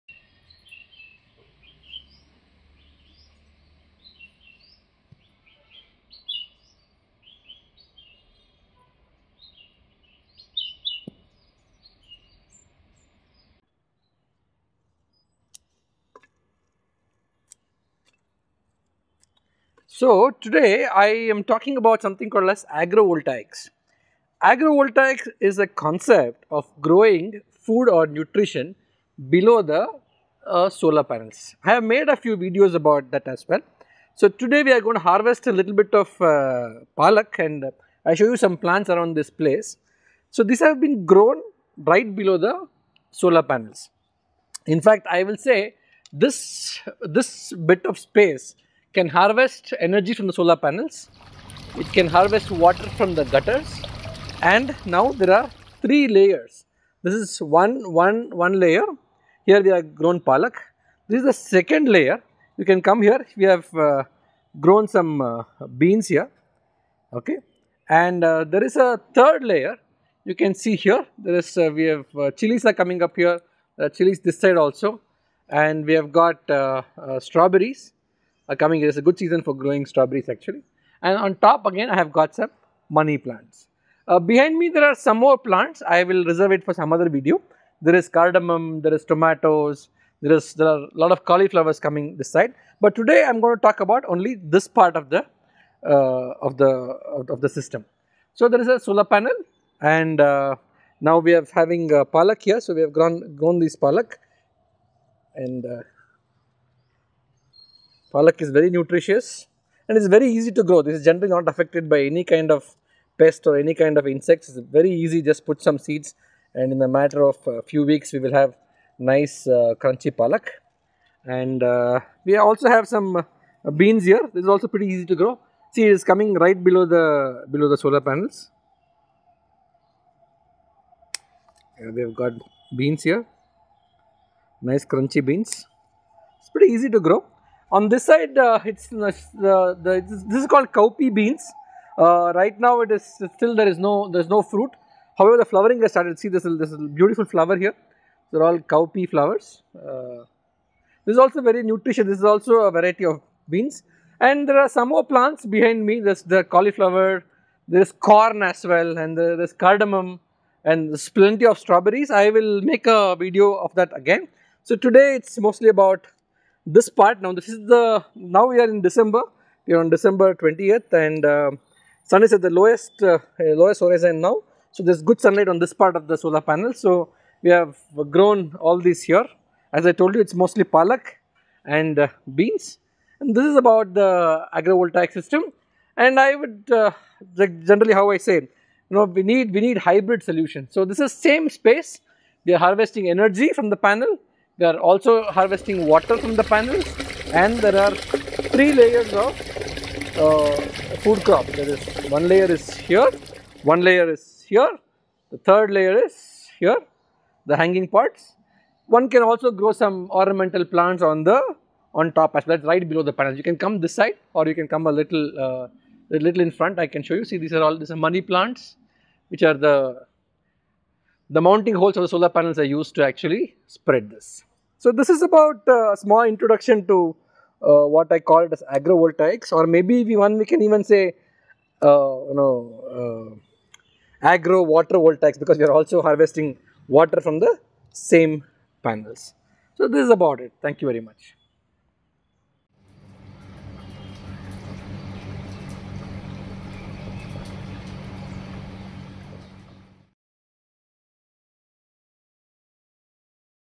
Agro-Hydro-Voltaics
Simple, implementable hybrid models can be one of the ways to be a little sustainable and to fight climate change at the individual level.
Here is one such example . This is popularly known as Agrivoltaics . I would go a step further and call it "Agro-hydro-Voltaics" .

The design is a simple elevated mounting structure for the Solar panels below which some food crops are grown . A gutter is installed to collect the Rainwater which can be used for domestic purposes.

It is called a hybrid system since the same space is used for harvesting energy , water and nutrition.

A 500 watt solar panel can yield about 700 units of clean energy and 2500 liters of clean water every year in a city like Bangalore.

Foliage Food crops like Cabbage , Cauliflower, Palak, Corn etc. yield better in the presence of differential/scattered sunlight and slightly higher humidity below the solar panels.
The sweating of leaves cools the solar panels thereby increasing its yield (energy output) . (A case study is given in the comments section)
agropv or agrivoltaics is also a good answer to the food vs fuel debate.

This kind of hybrid model completely de-couples the food-energy-water nexus achieving a higher degree of sustainability in all of them.

Energy generated from Rooftop has the least water foot-print and zero transmission and distribution losses .
Water generated from the rooftop has the least energy foot-print and zero transmission and distribution losses.
Nutrition harvested from the rooftop has least water and energy footprint and zero transmission and distribution losses .